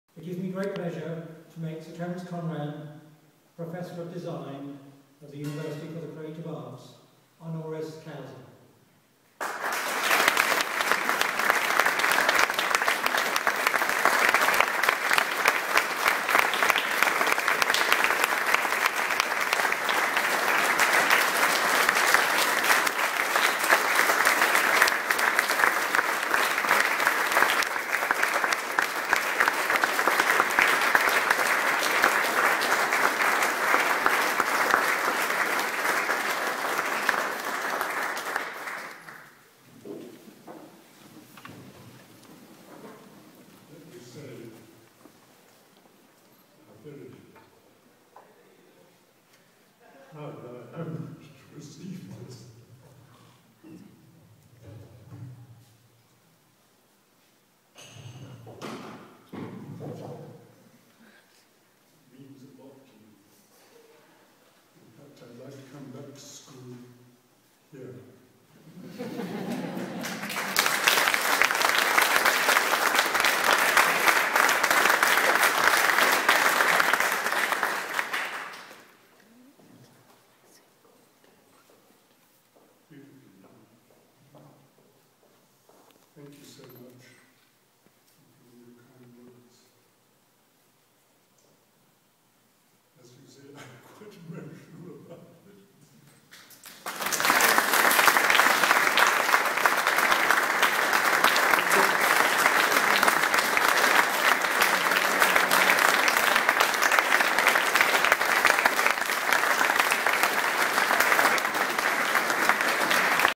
UCA ceremony as Sir Terence Conran becomes honorary professor
I was one of the 3D design students invited to the ceremony hence the video. Dr Simon Ofield-Kerr presented Sir Terence Conran with the honorary professorship. Click link for BBC article on the event.